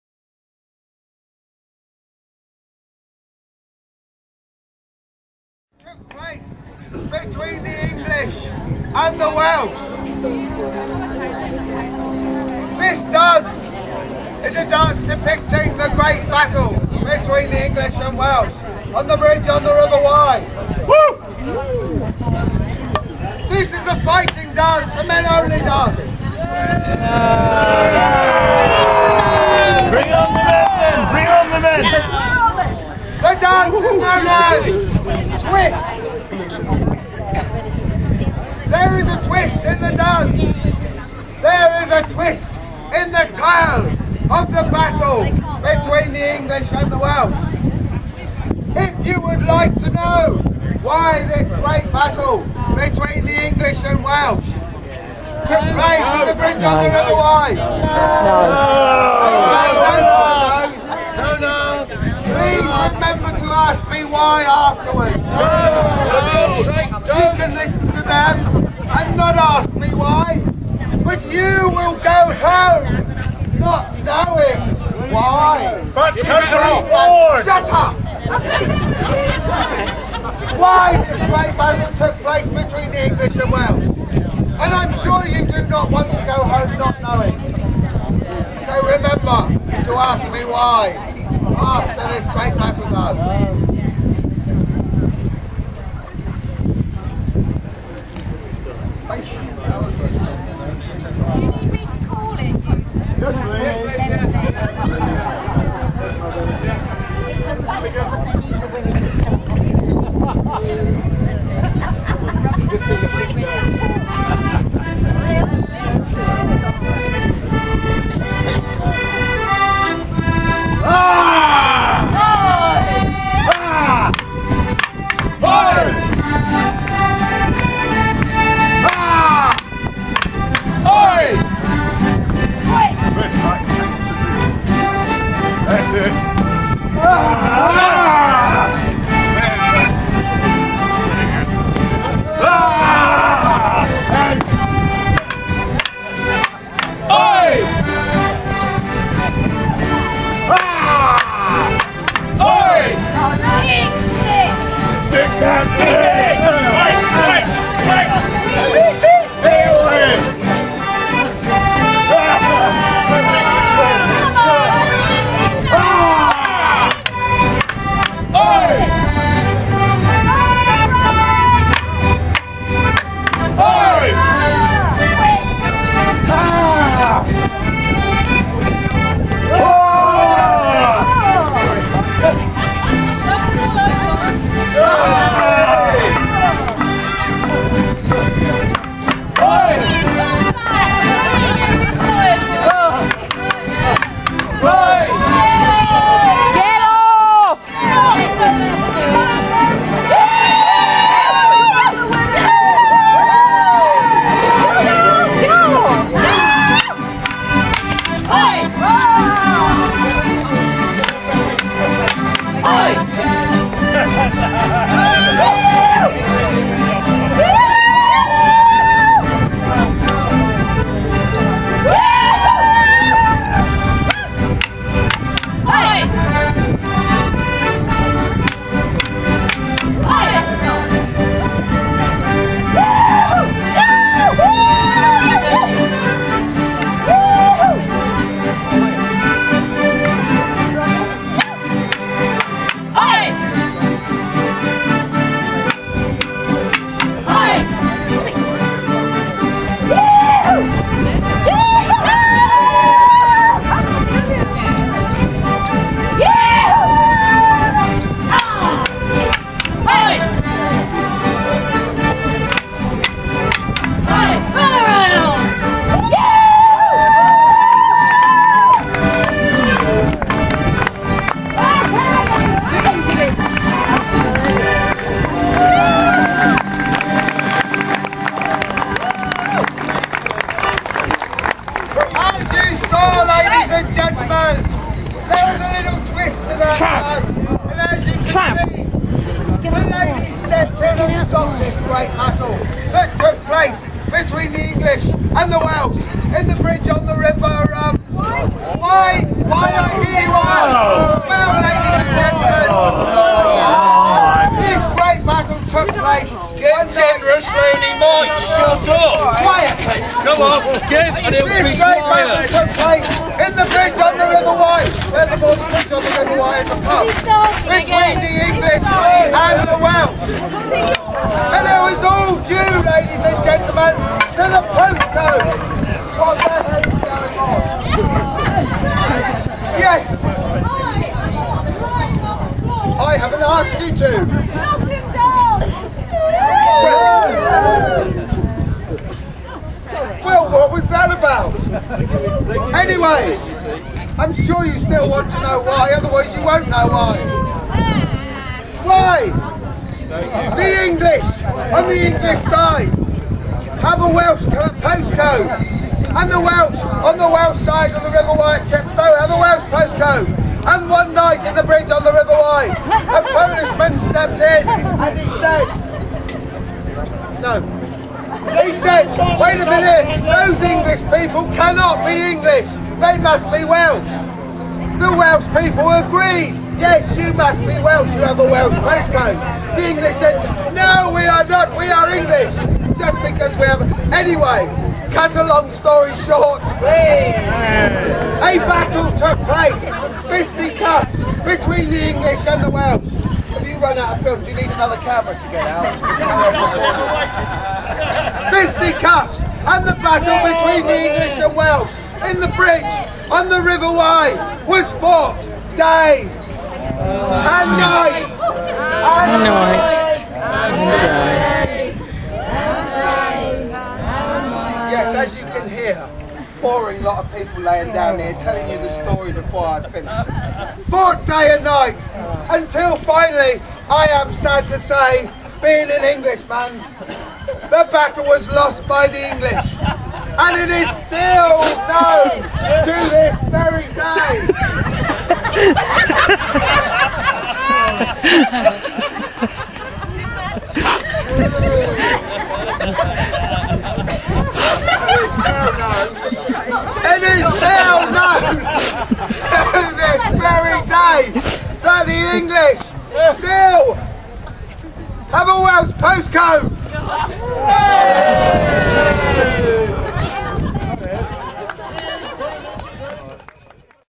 Twist with a Twist
Twist with a bit of fun. Shanklin seafront during Bloodstone Rally 2014